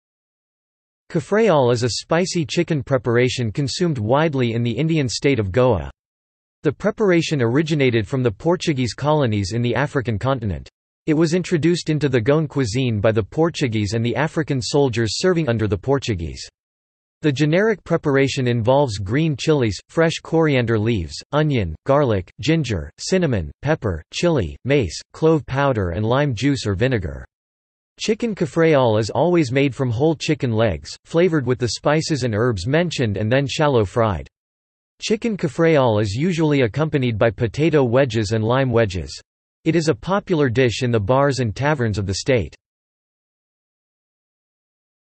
Cafreal | Wikipedia audio article
This is an audio version of the Wikipedia Article:
Cafreal

- Socrates

SUMMARY
Cafreal is a spicy chicken preparation consumed widely in the Indian state of Goa. The preparation originated from the Portuguese colonies in the African continent. It was introduced into the Goan cuisine by the Portuguese and the African soldiers serving under the Portuguese. The generic preparation involves green chillies, fresh coriander leaves, onion, garlic, ginger, cinnamon, pepper, chilli, mace, clove powder and lime juice or vinegar. Chicken Cafreal is always made from whole chicken legs, flavoured with the spices and herbs mentioned and then shallow fried. Chicken Cafreal is usually accompanied by potato wedges and lime wedges. It is a popular dish in the bars and taverns of the state.